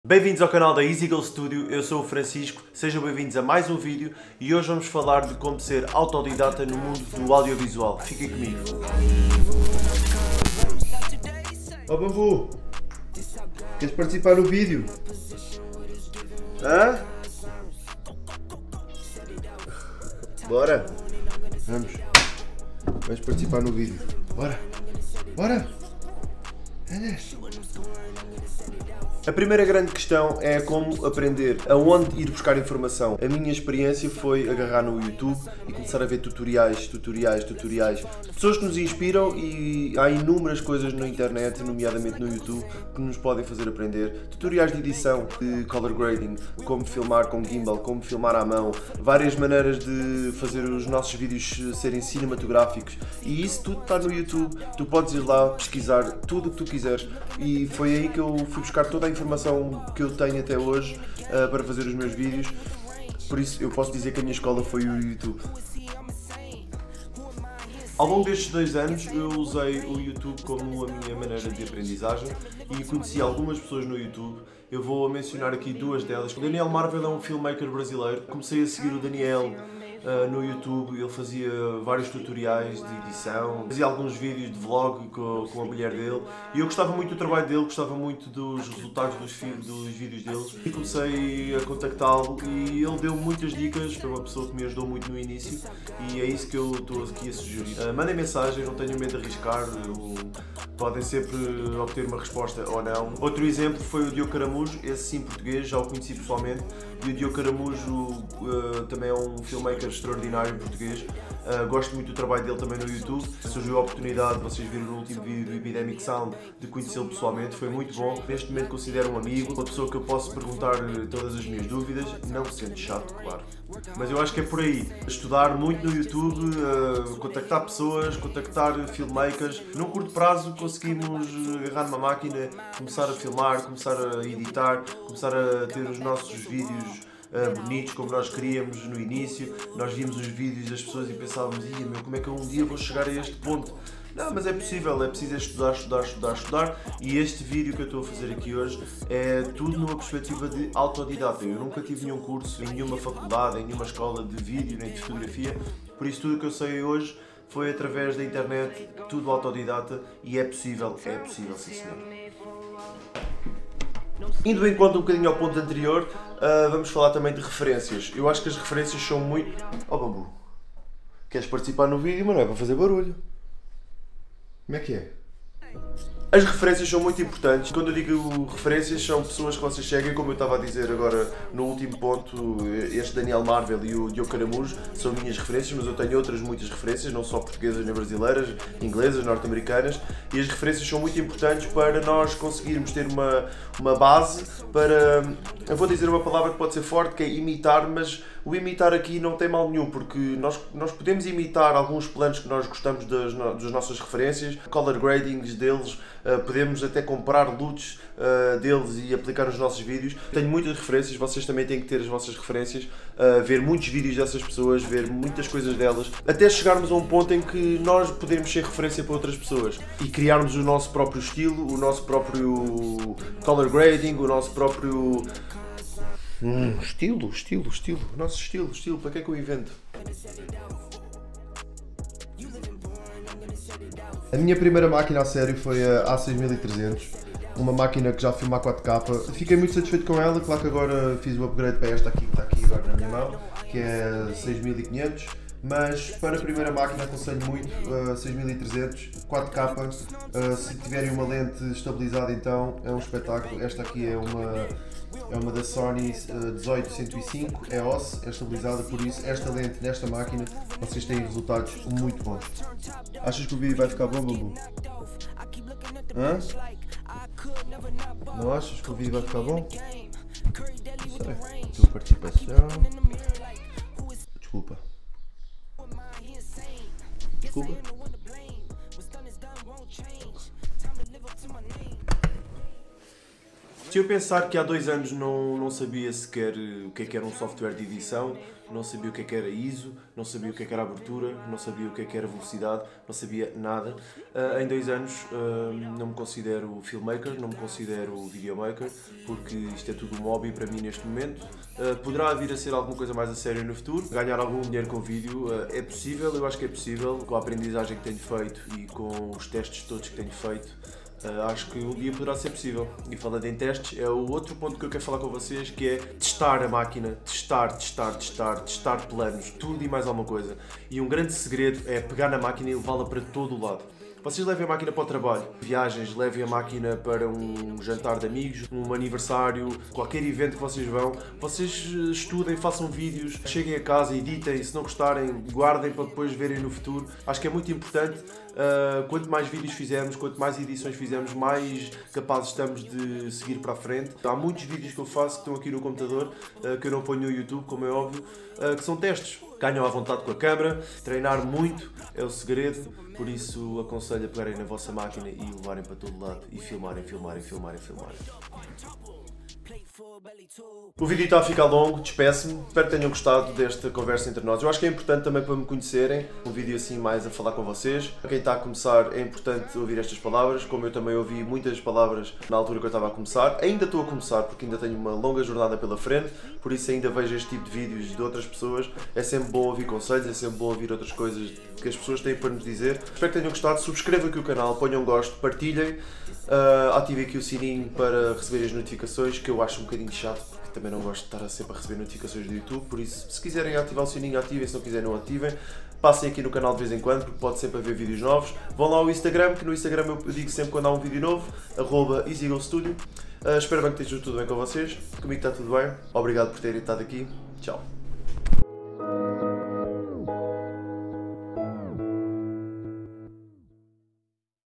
FILMAKER AUTODIDACTA
Decidi partilhar a minha experiência de como em dois anos consigo fazer vídeos tanto para o Youtube como a nível profissional, sendo eu um autodidacta.